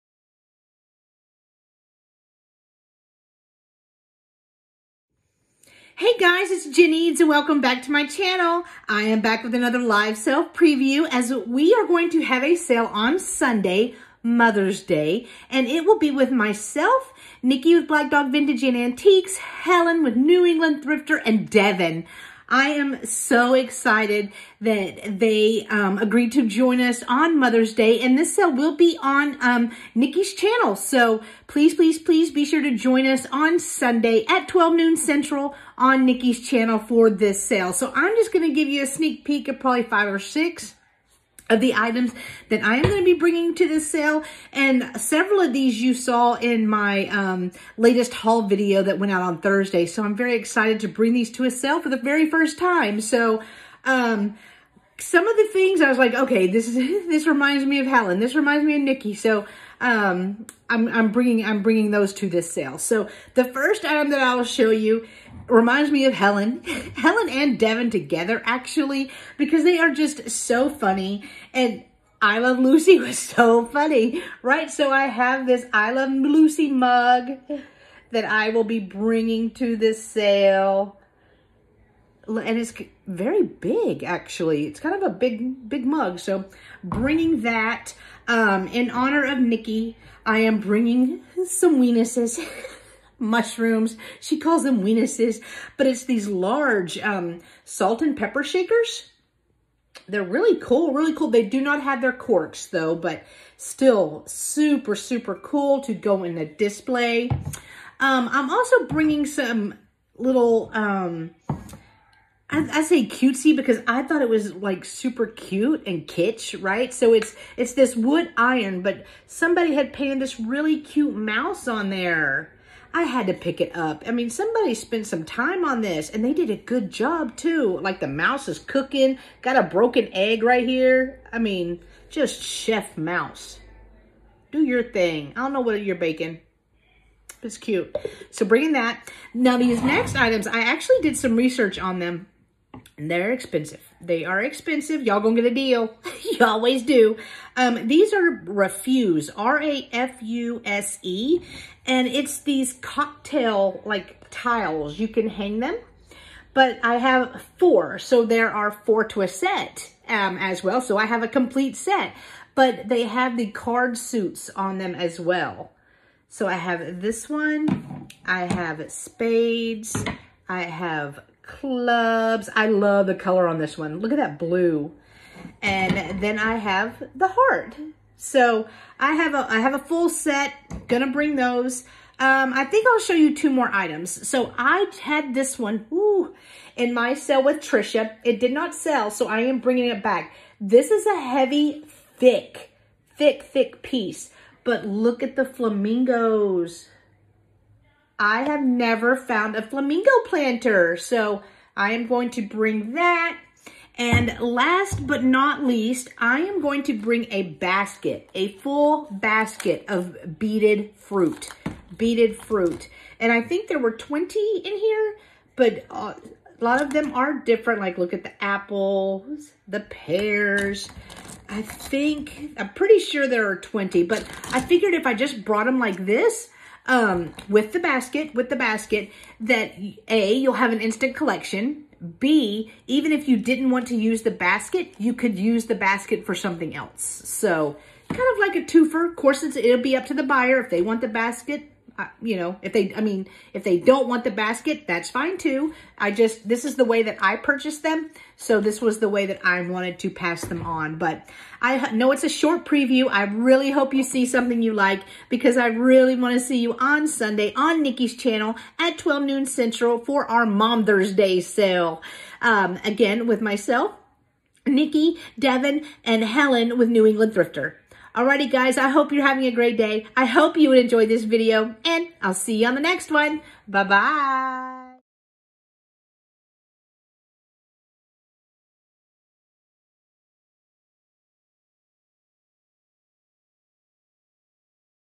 Mother’s Day Live Sale
Spend your Mother's Day with myself, Nikki, Helen and Devin! This sale will be on Nikki's channel.